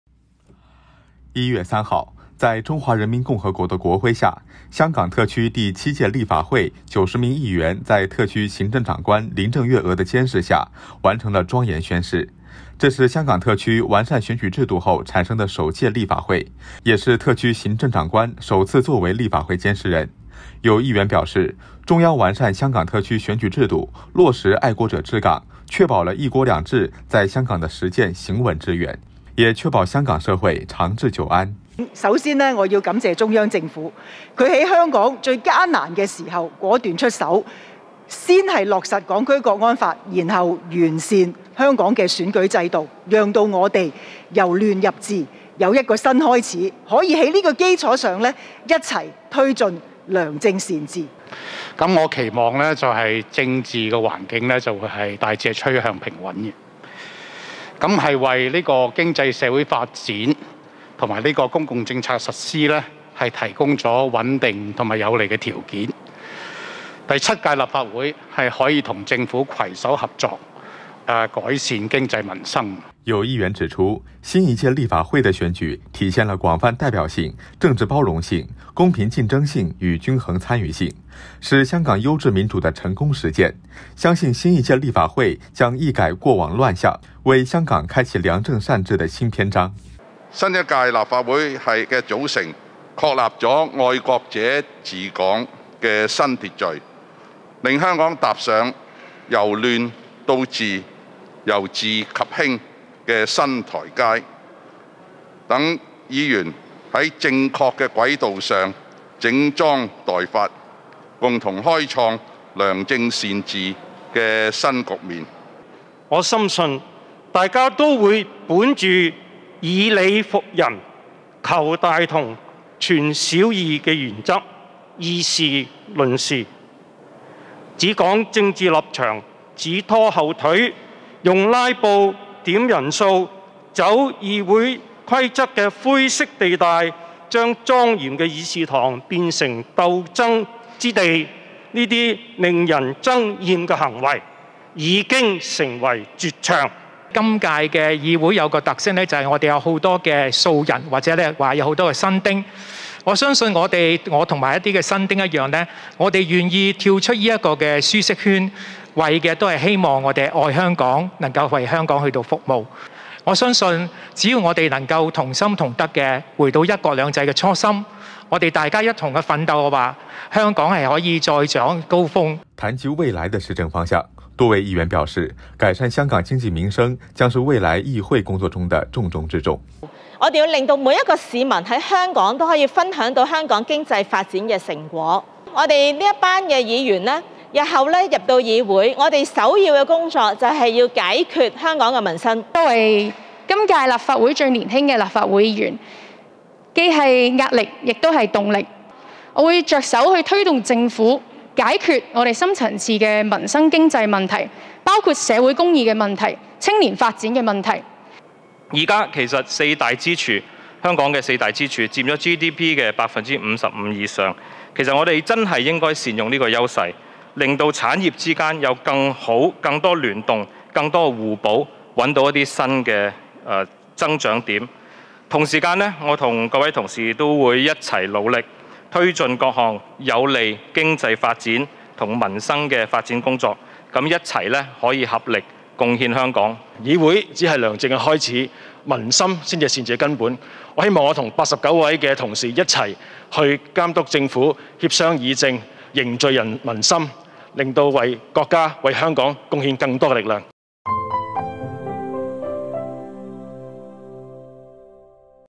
香港第七届立法会举行议员宣誓 议员冀改善经济民生
1月3日，在中华人民共和国国徽下，香港特区第七届立法会90名议员在特区行政长官林郑月娥的监誓下，完成了庄严宣誓。这是香港特区完善选举制度后产生的首届立法会，也是特区行政长官首次作为立法会监誓人。有议员表示，中央完善香港特区选举制度，落实“爱国者治港”，确保了“一国两制”在香港的实践行稳致远，也确保香港社会长治久安。

有议员指出，新一届立法会的选举体现了广泛代表性、政治包容性、公平竞争性与均衡参与性，是香港优质民主的成功实践。相信新一届立法会将一改过往乱象，为香港开启良政善治的新篇章。

谈及未来的施政方向，多位议员表示，改善香港经济民生将是未来议会工作的重中之重。（范思忆 罗嗣昱）